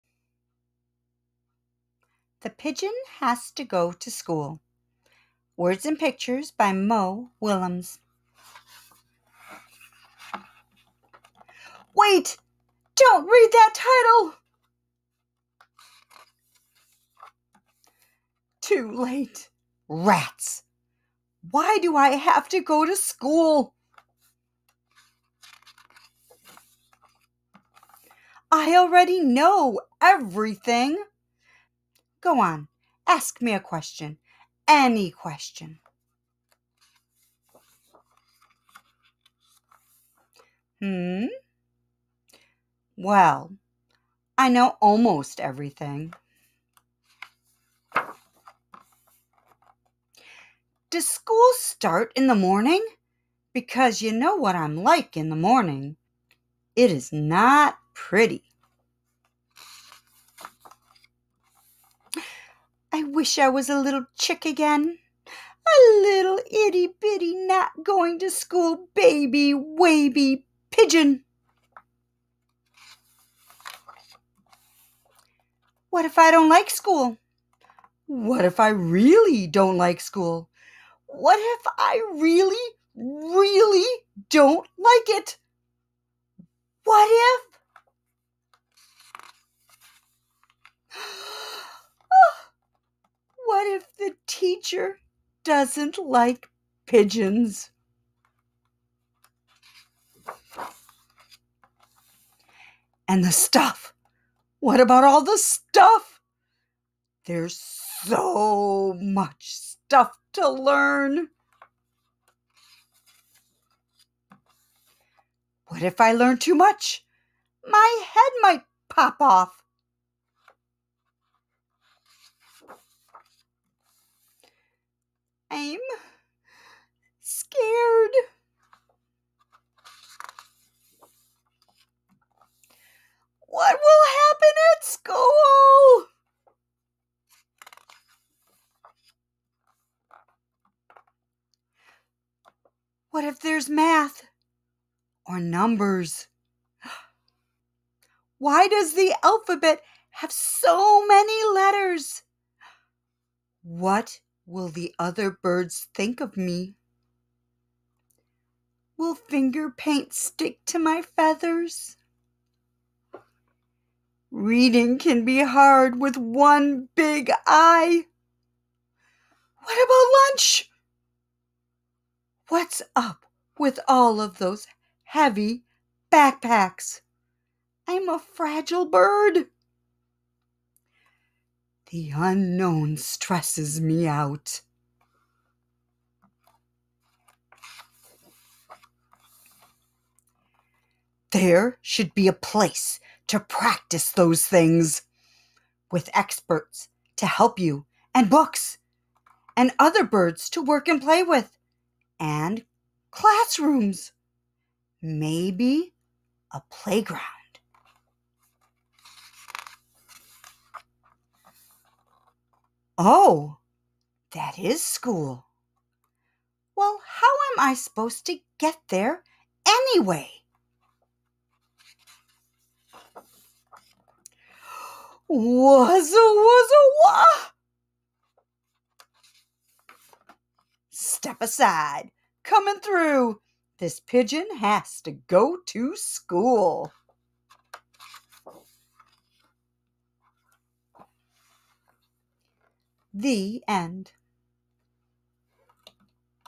The Pigeon HAS to Go to School!
Funny story about starting school.